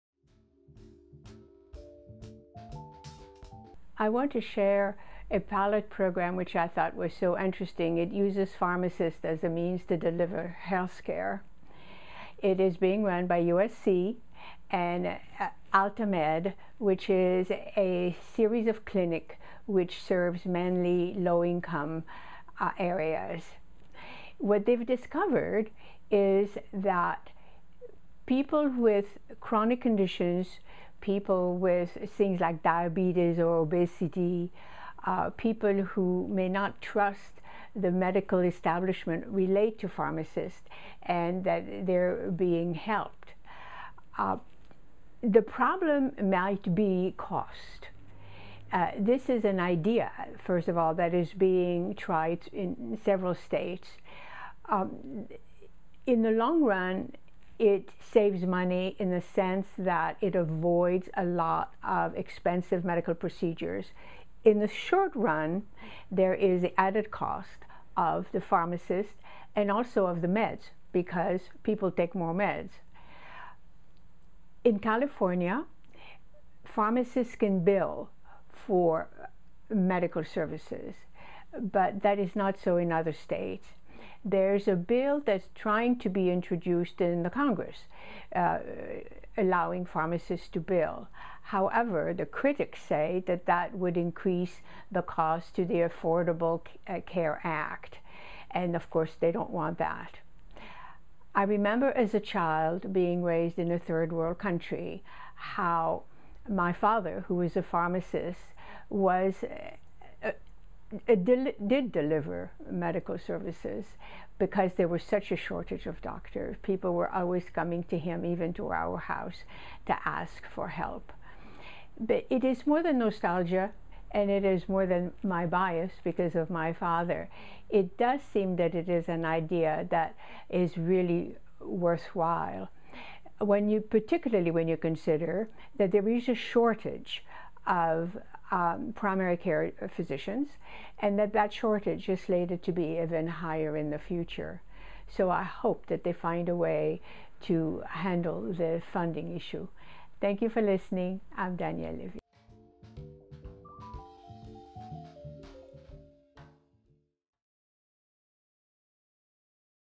pharmacists
A pilot program with promising results uses pharmacists to fill in a gap in delivering medical services.